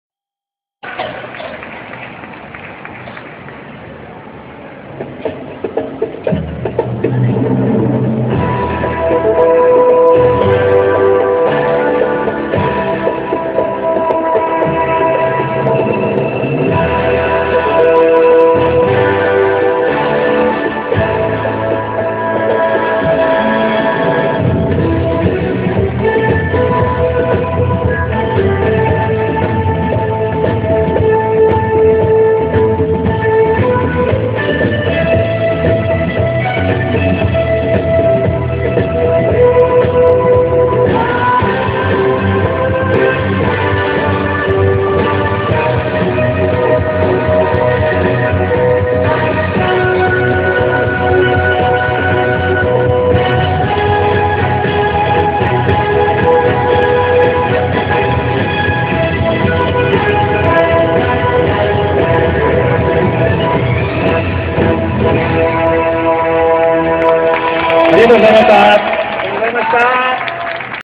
栄オアシス21／田代圭『筋肉祭 鍛えたカラダは美しい』フリーポーズ
テレビ愛知開局30周年イベント「筋肉祭、鍛えたカラダは美しい!!」日時：5/5(日)  11:00～11:30  場所：栄オアシス21特設ステージby愛知県ボディビル,フィットネス連盟